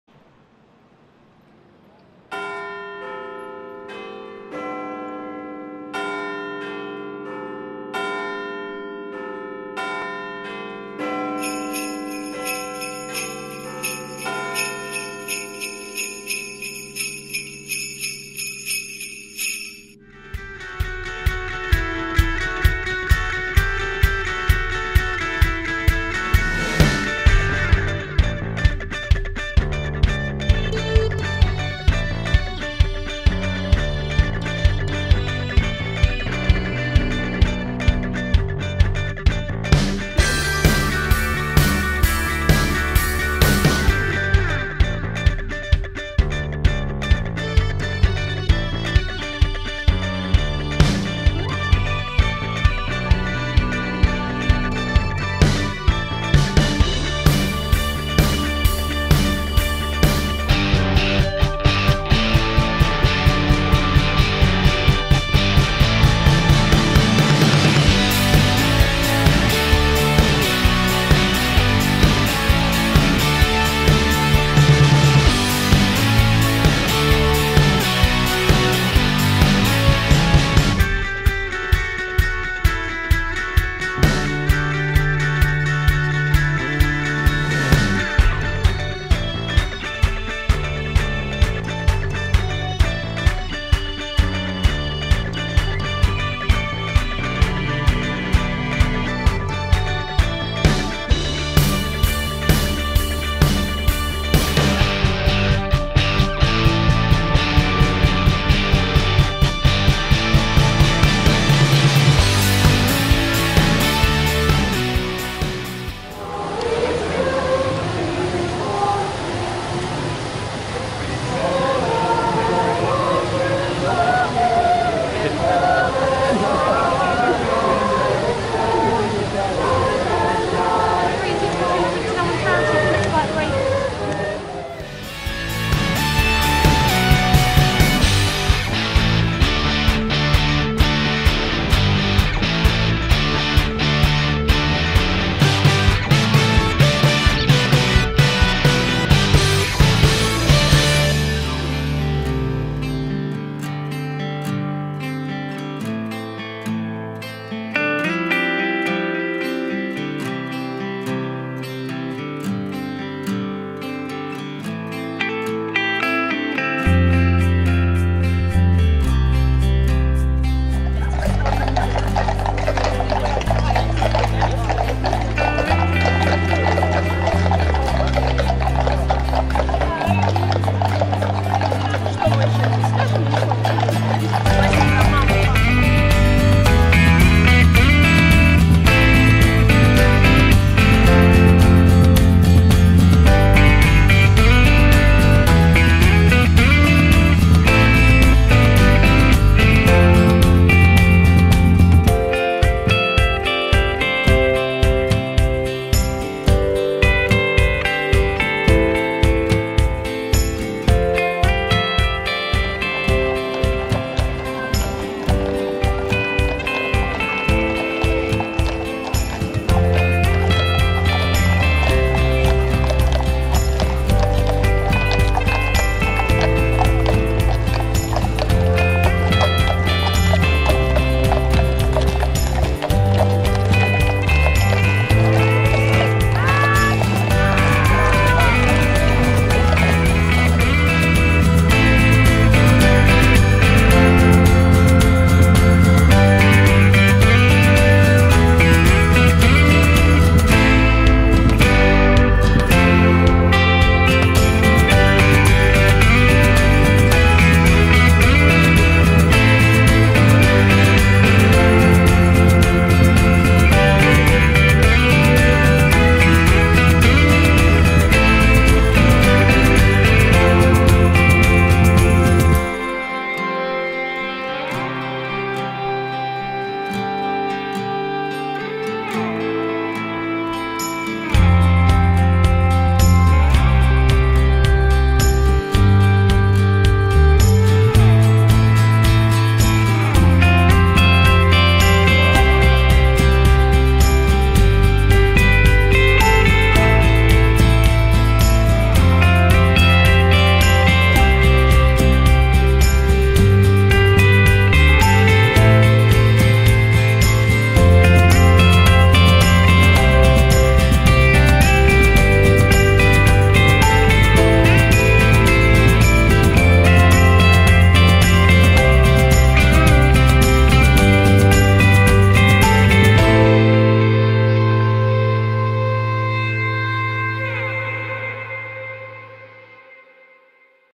05 London Day 2 Part 1. Trafalgar Square. The Mall. St James's Park.
A short visit to London on Christmas Eve 2018. Drawn by the spleen, but supported by the city foxes of Pimlico...

0:00 Intro
0:20 Pimlico, early morning...
0:50 Trafalgar square
2:30 The Mall
4:05 St James's Park
5:14 Are you still here?